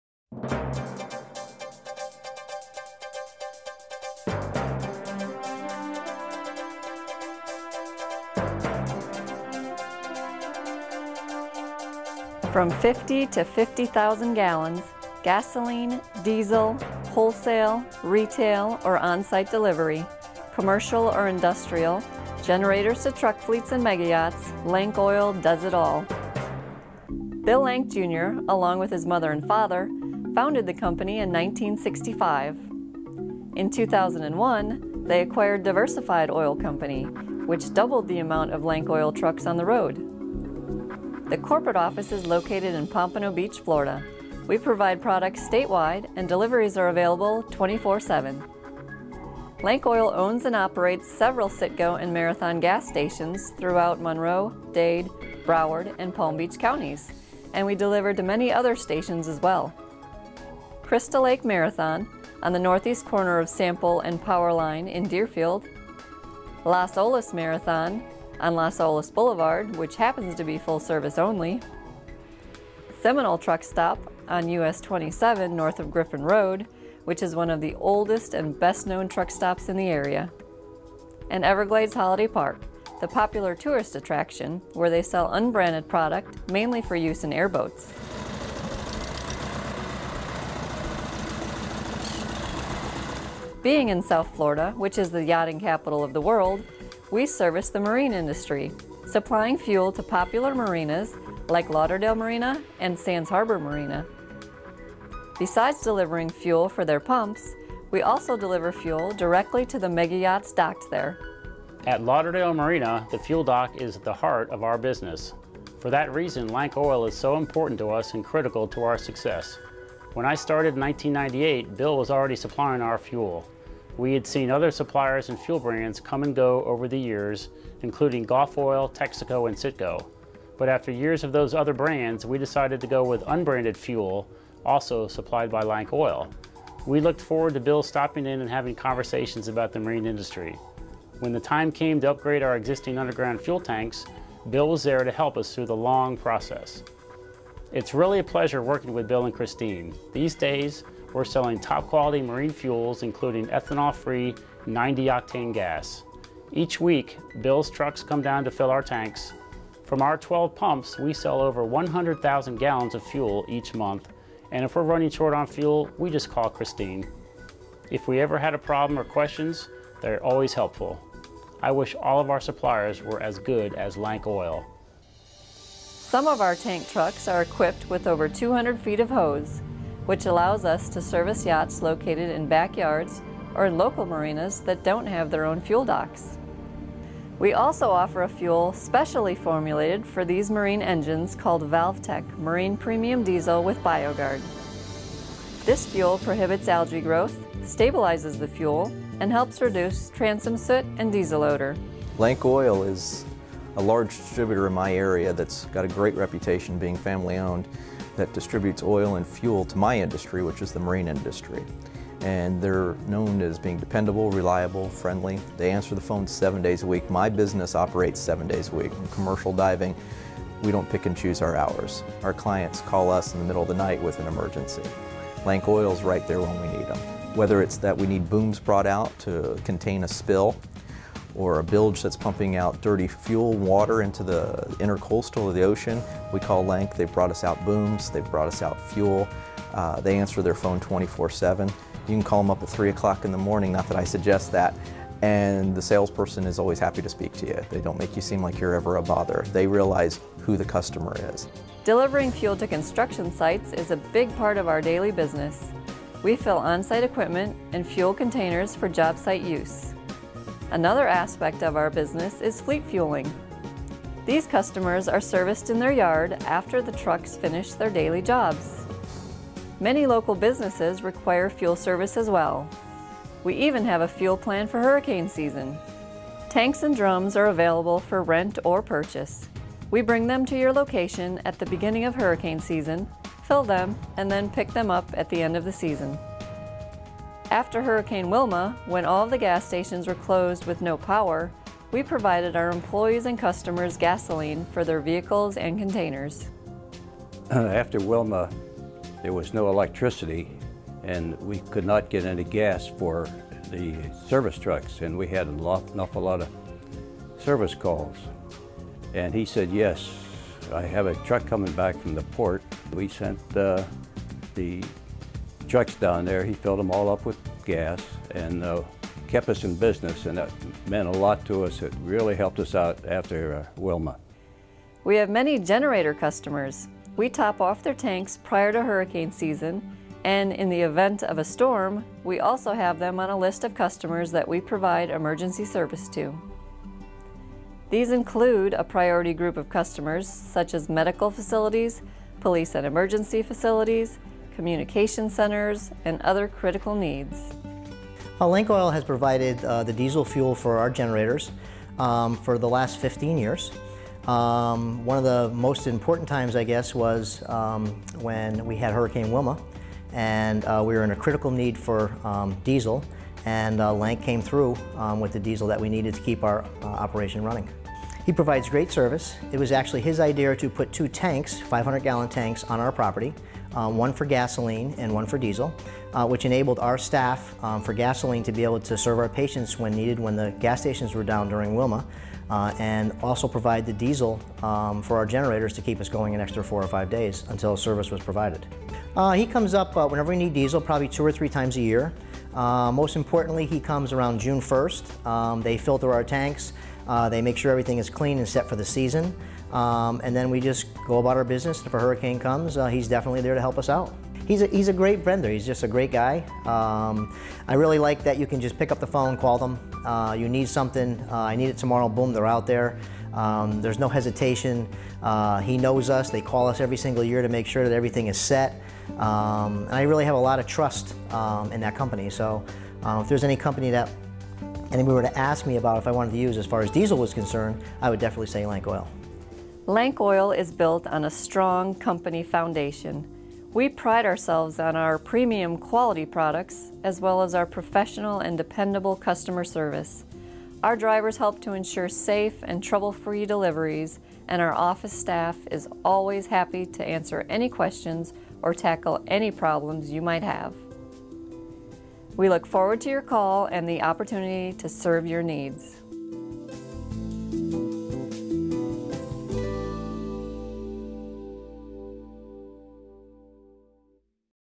Lank Oil 2012 Corporate Story
Kristene Lundblad, Director of Sales and Marketing gives a 2012 overview of Lank Oil's Services